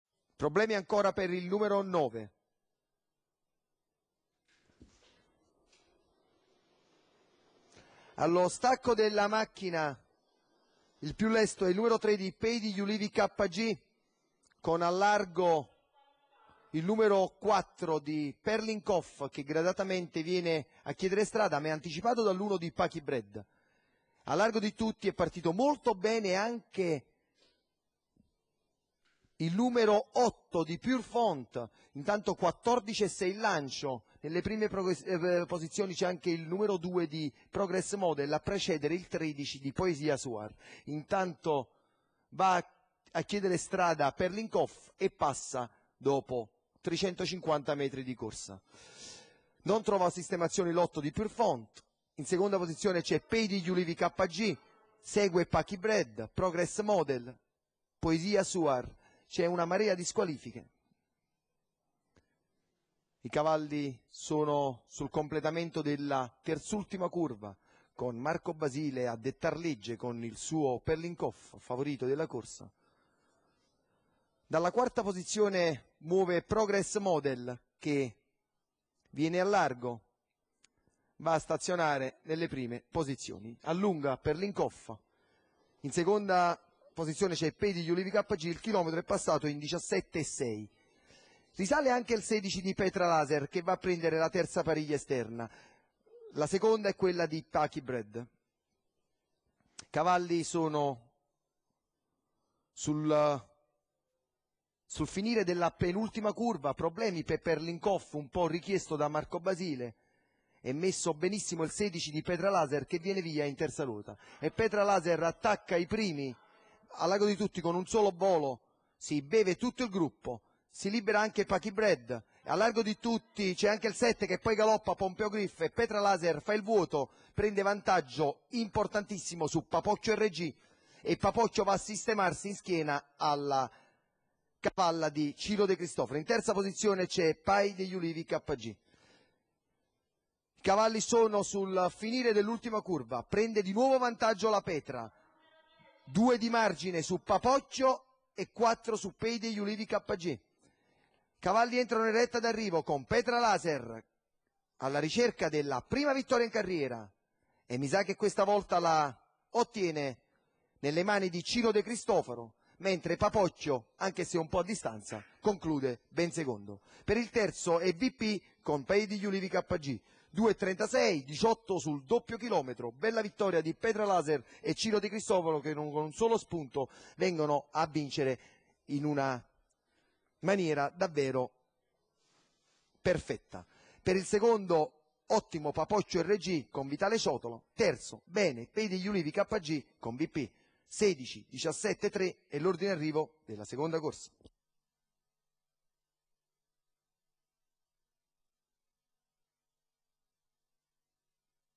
21/09/2012, Foggia , Premio Nebbiolo
1. PETRA LASER (DE CRISTOFARO C.), 2. PAPOCCHIO RG (CIOTOLA VITALE), 3. PAY DEGLIULIVI KG (DELL' ANNUNZIATA V P), 4. PARTANNA BI (D' ALESSANDRO GIORGIO JR), 5. PERITAS (GOLIA ATTILIO), 6. PACKY BREED (CINCINNATO FR.), 7. PHOEBY CANF (FORINO RAFFAELE), 8. PERLIN COF (BASILE M.), 9. PATERNO DI LIMA (GALLUCCI ROMEO), 99. PROGRESS MODEL (TRINCHILLO G.), 99. PINO SAURO (BORRINO SALVATORE SR), 99. POMPEO GRIF (BEVILACQUA S.), 99. PURE FONT (RICCIO GEN.), 99. PREZIOSOSAN (D' ALESSANDRO VINCENZO), 99. PEDROPER (SCHERILLO GENNARO), 99. POESIA SUAR (DI NARDO GAETANO)